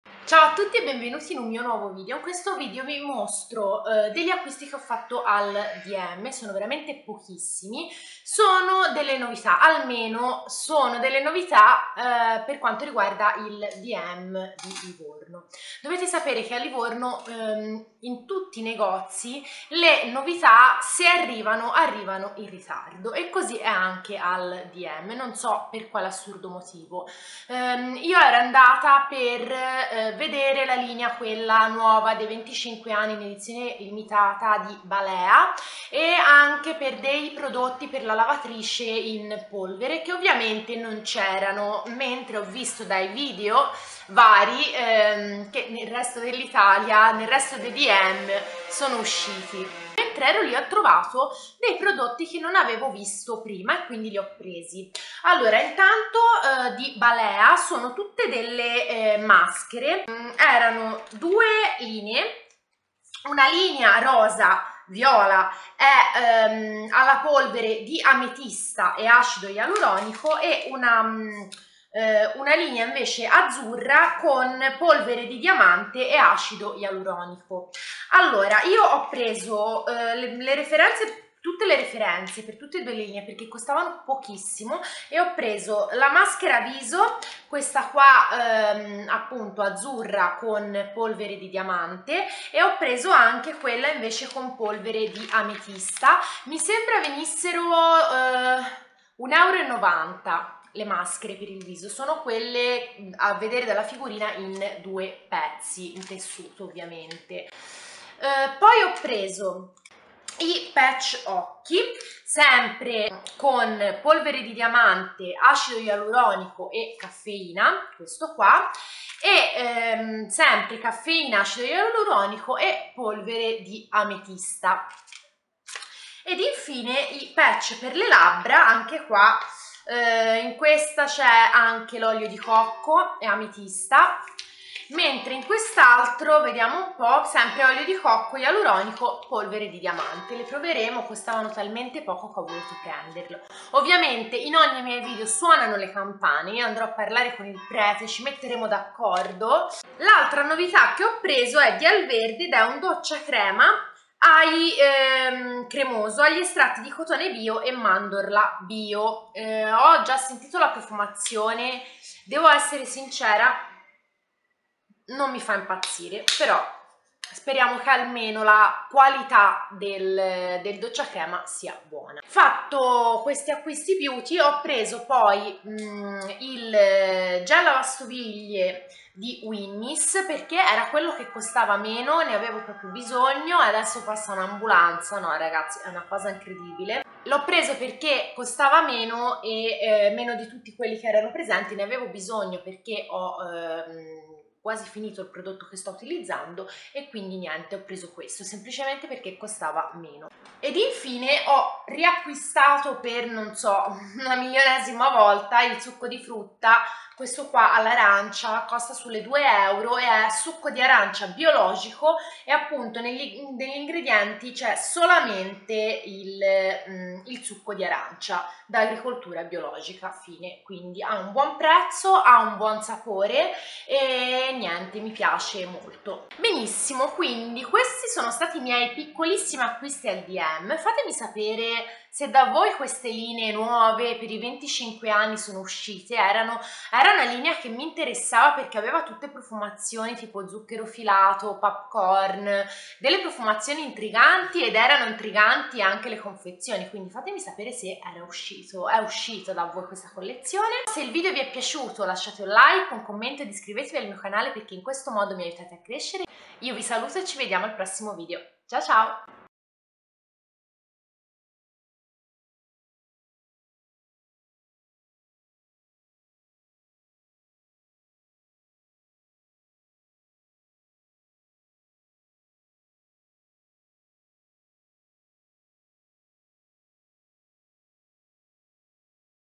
Acquisti novità Dm Markt
In questo video ti mostro le novità che ho acquistato da Dm Markt

Qui sotto potete trovare i post con alcuni prodotti citati nel video nel dettaglio (inci a pallini, info sul prodotto e recensione,...):